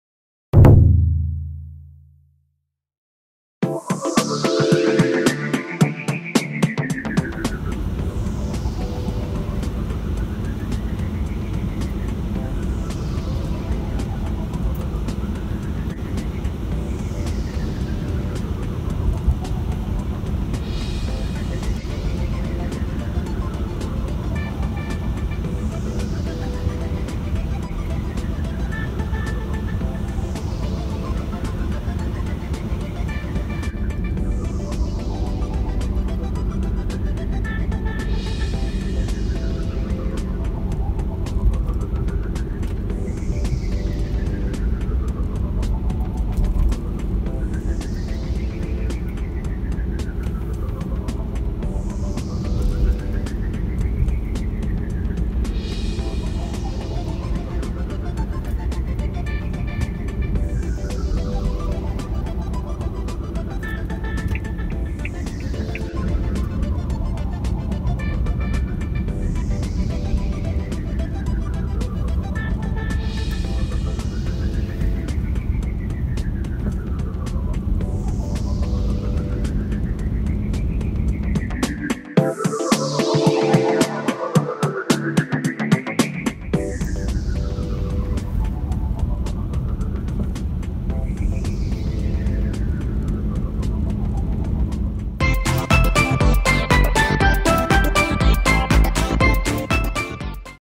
新型ジムニー横転事故【深夜1時ブラックアイスバーン路面】
外気温マイナス1度で深夜1時のツルツル路面
ブラックアイスバーンで新型ジムニーが横転事故していました

よりによって警察署目の前です
スピードの出しすぎには注意しましょう